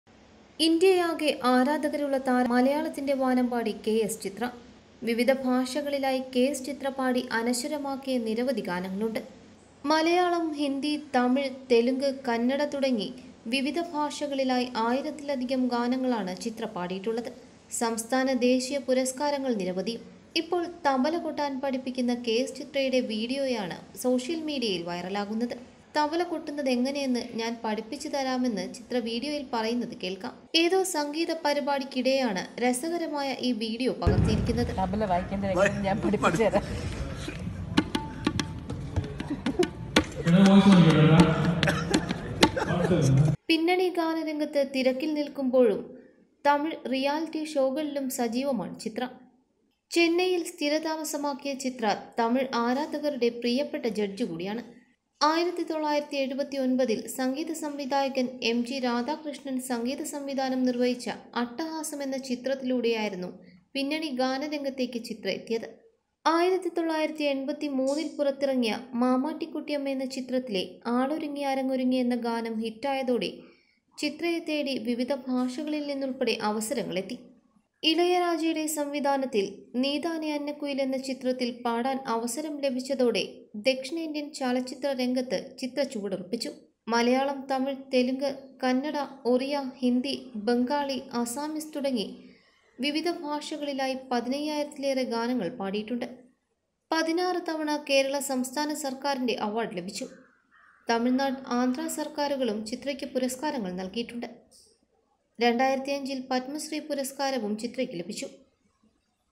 ആശാനെ തബല കൊട്ടാന്‍ പഠിപ്പിച്ച് ചിത്ര
വാനമ്പാടി ചിത്രയുടെ ഒരു വീഡിയോയാണ് സോഷ്യല്‍ മീഡിയയില്‍ ശ്രദ്ധ നേടുന്നത്.